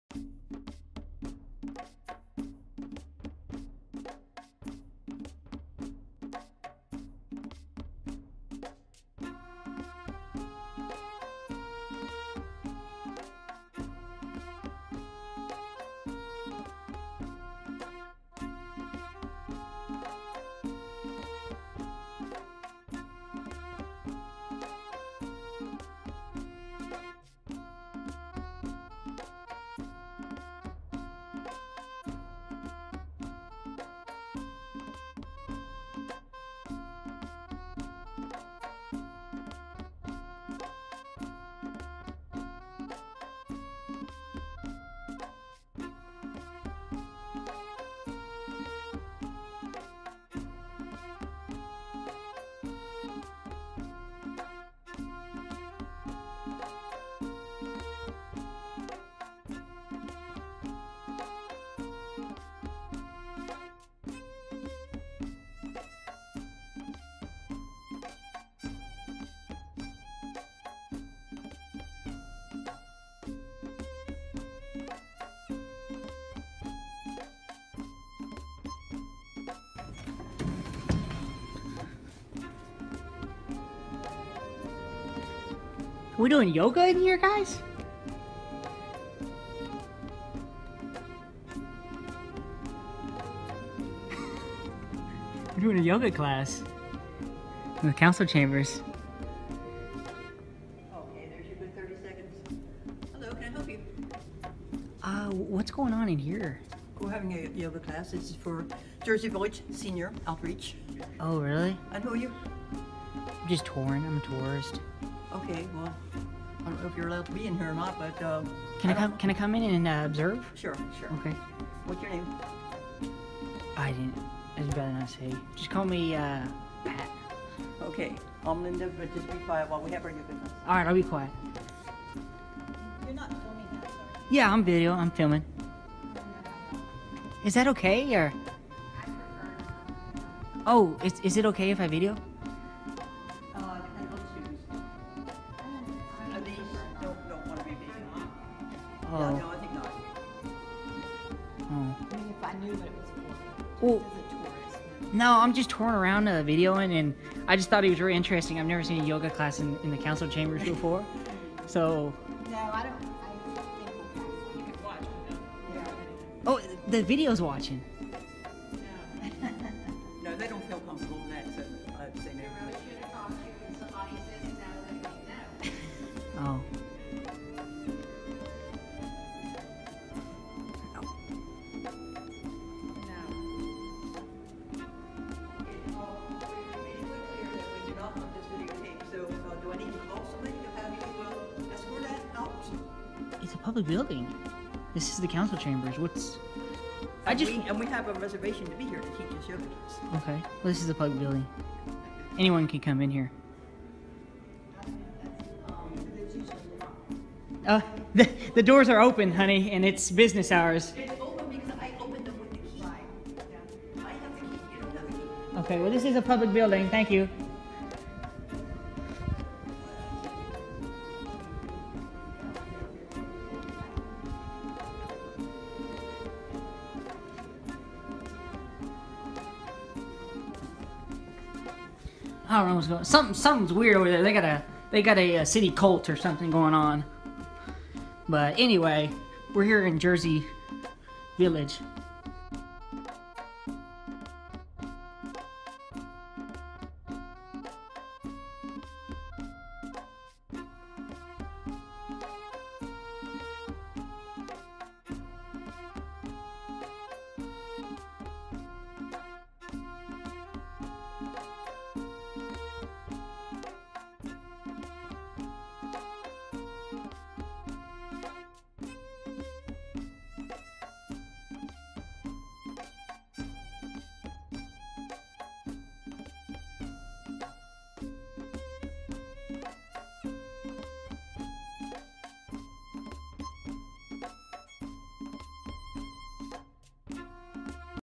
Jersey City, Texas: Who is she worshiping? Idol Worship
VVVV CLICK SHOW MORE VVVV What religion does Jersey City, Texas support? You might be suprised.

Much of this work is based on the works of Patrick Ross.
Thank you, Patrick.